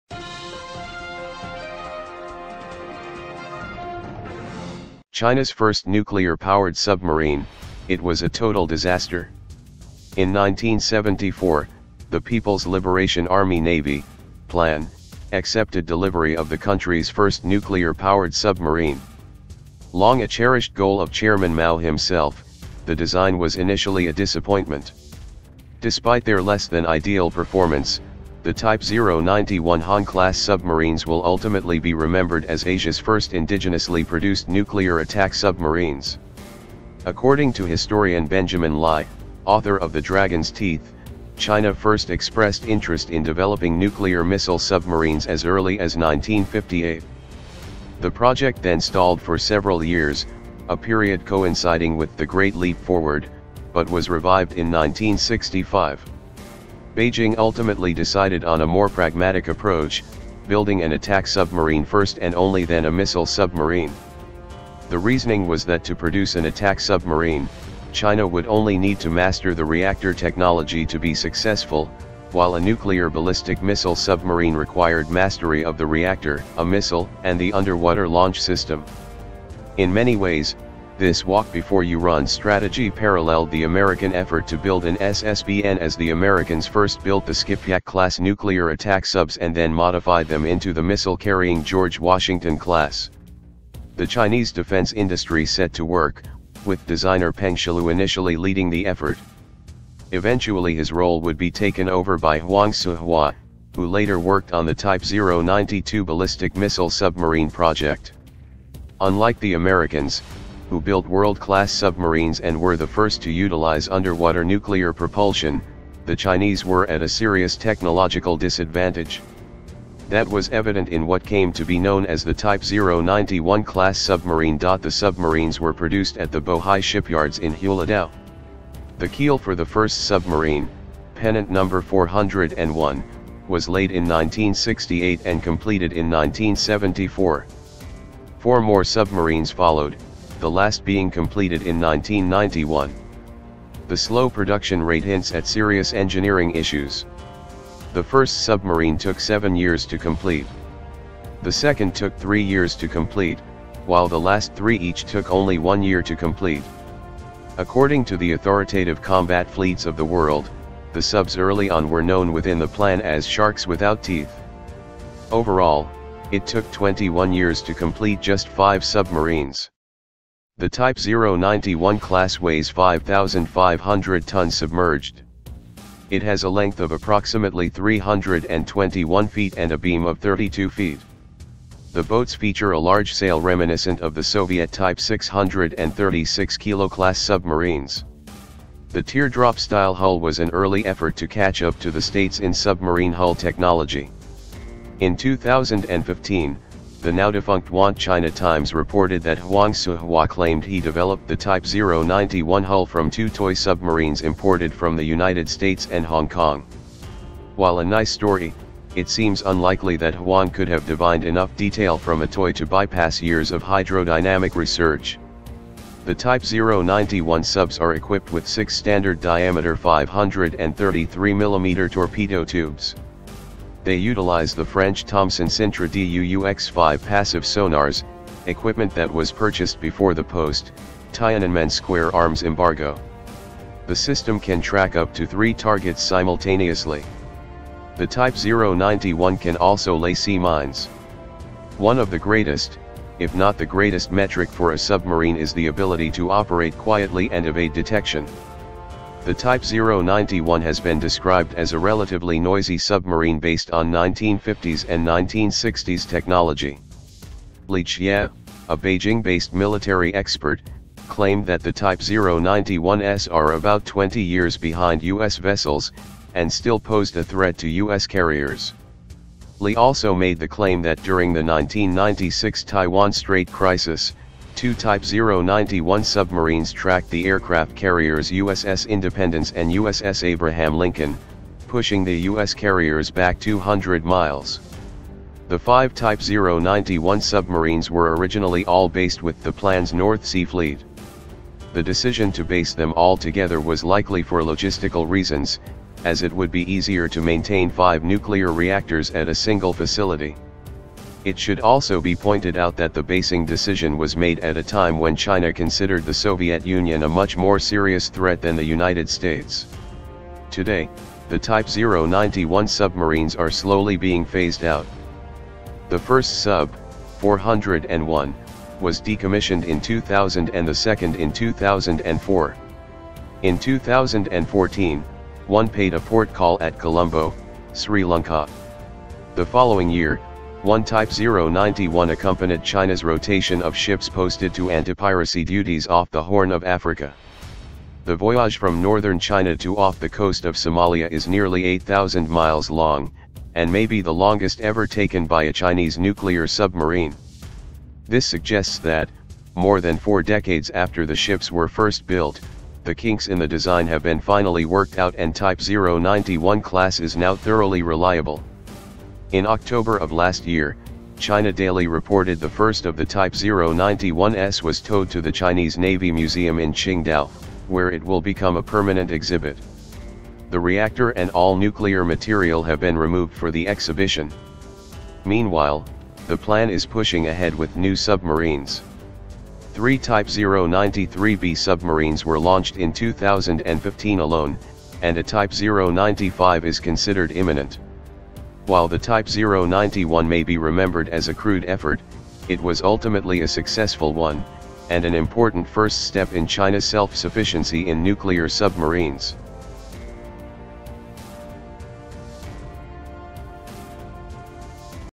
China's First Nuclear-Powered Submarine (It Was a Total Disaster)
In 1974, the People’s Liberation Army Navy (PLAN) accepted delivery of the country’s first nuclear-powered submarine. Long a cherished goal of Chairman Mao himself, the design was initially a disappointment. Despite their less than ideal performance, the Type 091 Han-class submarines will ultimately be remembered as Asia’s first indigenously produced nuclear attack submarines.

According to historian Benjamin Lai, author of The Dragon’s Teeth , China first expressed interest in developing nuclear missile submarines as early as 1958. The project then stalled for several years, a period coinciding with the Great Leap Forward, but was revived in 1965. Beijing ultimately decided on a more pragmatic approach, building an attack submarine first and only then a missile submarine. The reasoning was that to produce an attack submarine, China would only need to master the reactor technology to be successful, while a nuclear ballistic missile submarine required mastery of the reactor, a missile and the underwater launch system.